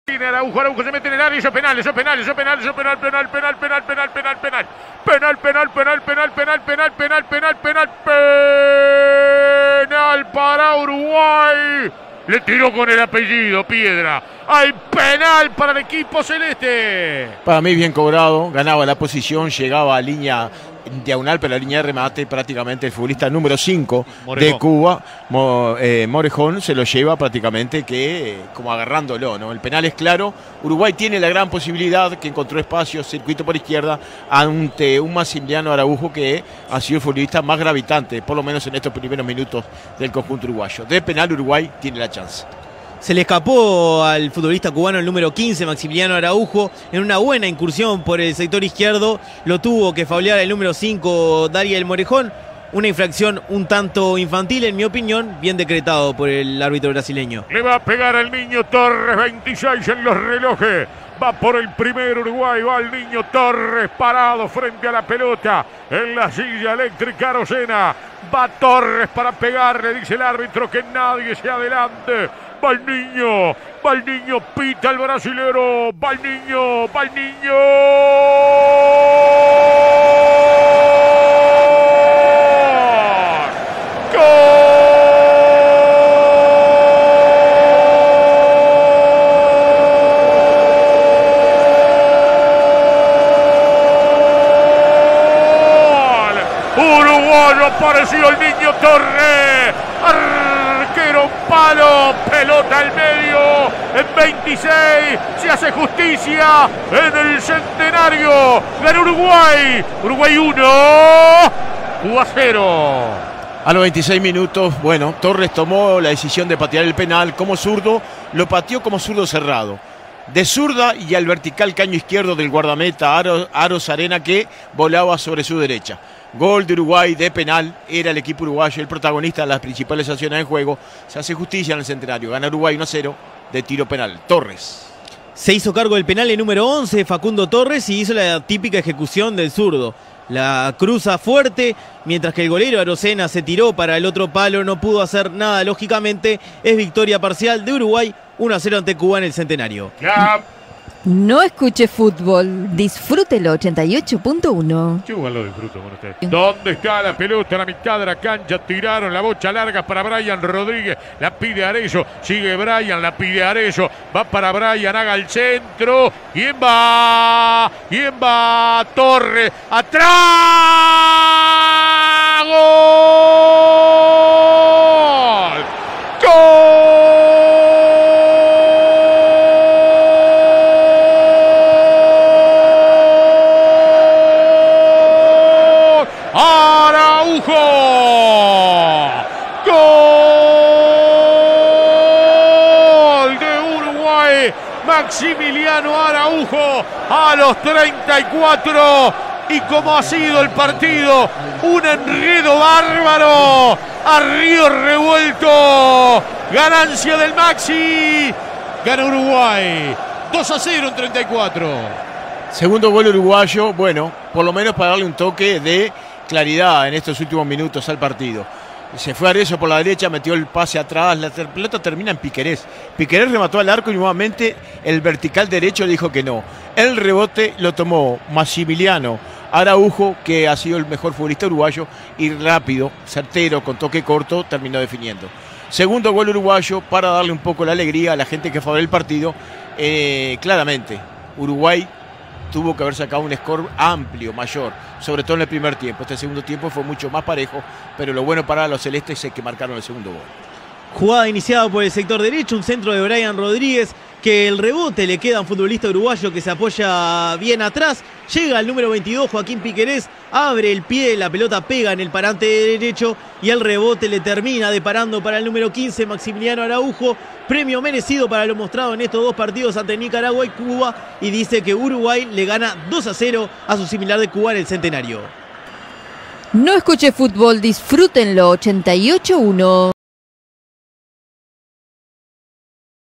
Uruguay 2 Cuba 0 - Amistoso (por Luis Morales)
Los goles del partido, por Luis Morales junto a Leonardo Amarilla y Franco Abreu para 88.1 FM Punta del Este.